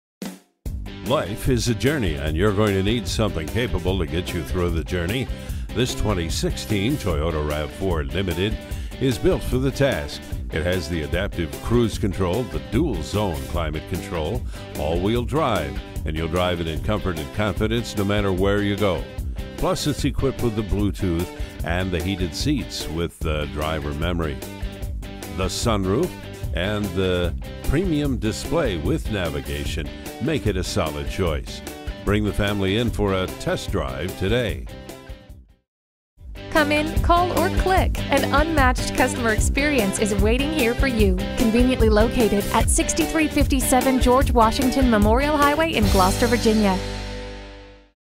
New 2016 Toyota RAV4 Gloucester VA Newport News, VA #7000 - SOLD
Year: 2016
Make: Toyota
Model: RAV4
Trim: Limited
Engine: 2.5L 4 cyls
Transmission: Automatic 6-Speed
Color: Magnetic Gray Metallic
Stock #: 7000
VIN: 2T3DFREV3GW435030

Gloucester Toyota offers a full selection of service areas including:
Address:
6357 George Washington Memorial HWY